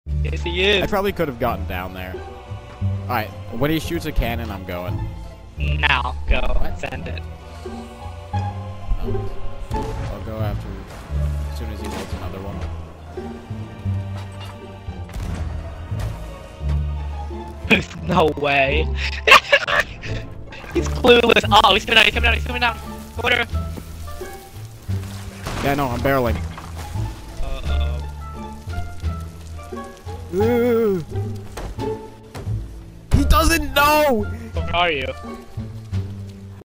bro did not see me
how can you miss the giant moving barrel in the bottom of your ship